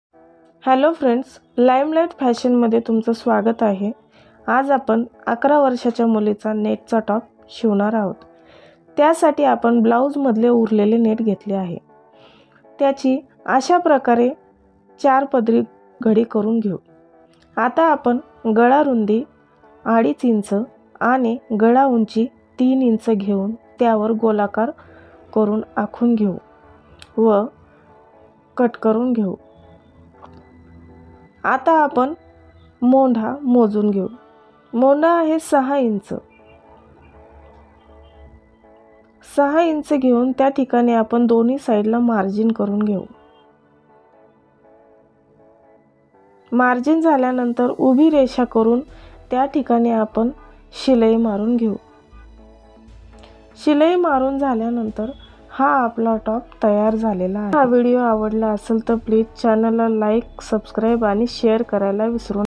easy net jaket/top stiching#shorts#
easy top stitching #

this is net top stich very easy to beginner learn silai work
this net jaket/top wearing any dress/top then dress look is very nice and beautiful
this jaket stiching baby girl top 11 year old required cloth 1/2 meter

top net jacket
measurements
hight-18
chest-23
west height-10
hip height-16
hip round-25
sholder-15
west round-23